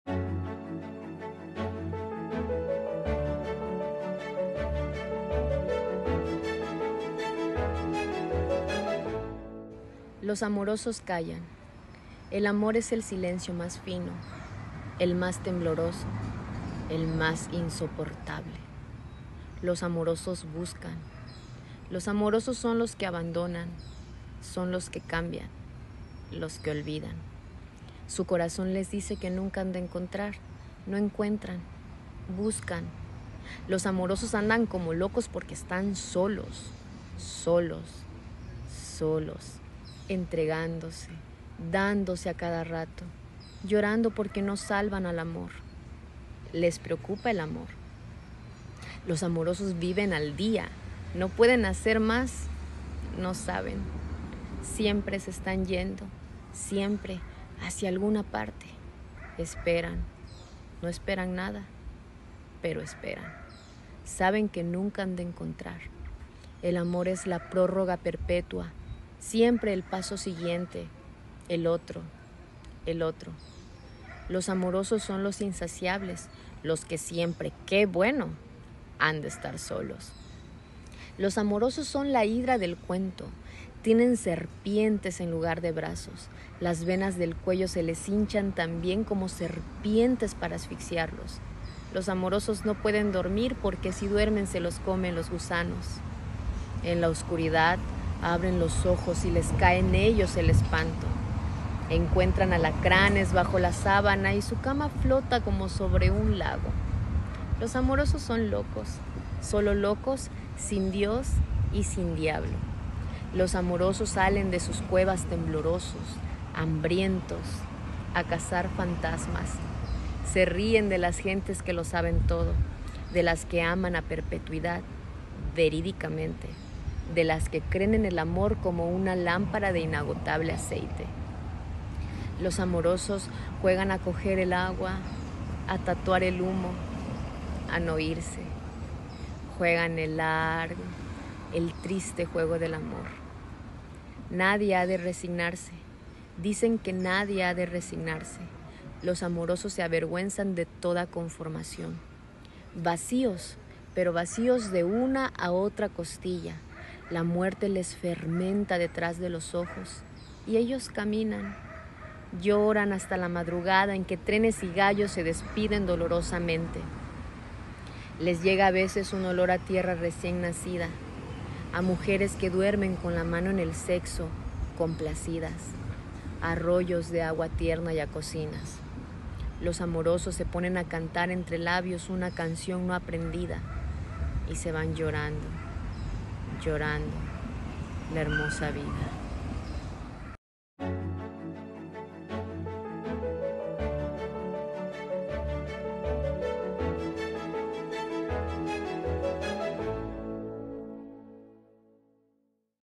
Programa "Hacia una Universidad Lectora". Los Amorosos, Jaime Sabines.
En el marco del Día Nacional del Libro, la Universidad de Ciencias y Artes de Chiapas, te invita a leer a través del programa "Hacia una Universidad Lectora".
Lectora: Osiris Trujillo Castellanos. Preuniversitario de la Facultad de Música UNICACH.